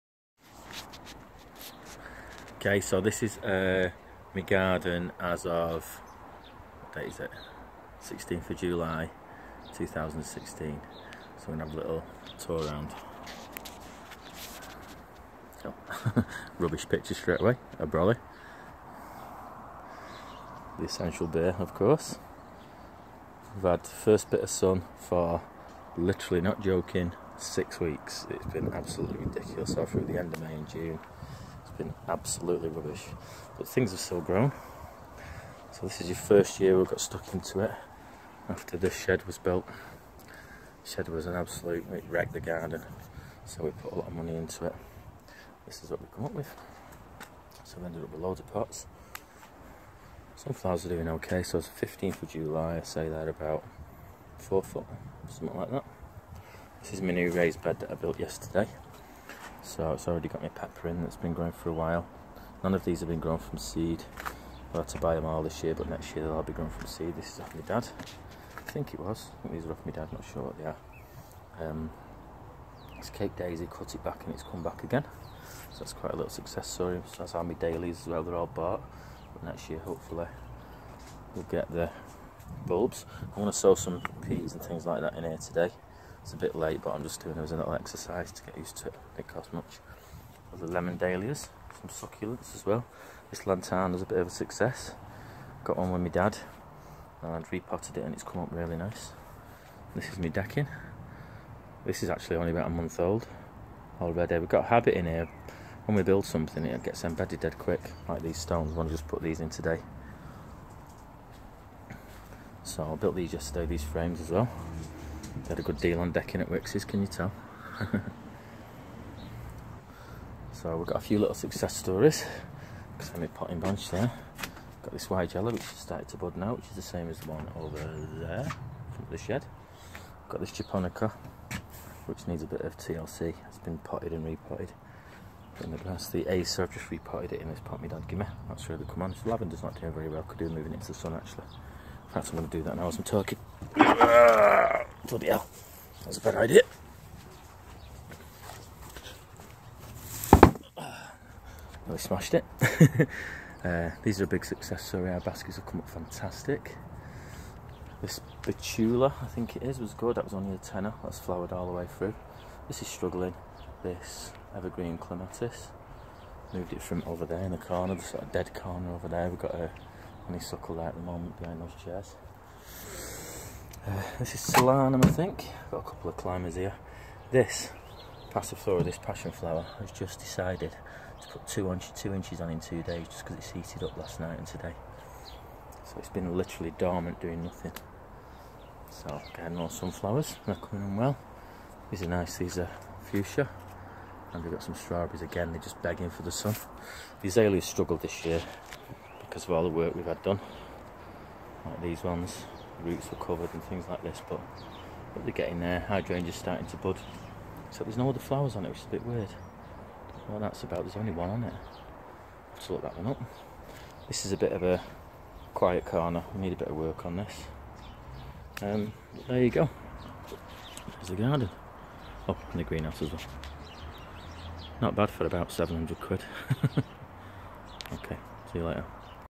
An English country garden in the middle of Salford
A quick tour of my little garden here in the middle of Salford in the North West of England. It was an absolute wreck 2 months ago after i built the shed at the back (actually a storage building for my business) but it's not looking too bad now.  I'm very much a beginner gardener so all thoughts and any corrections to the plant names i mention will all be greatly appreciated.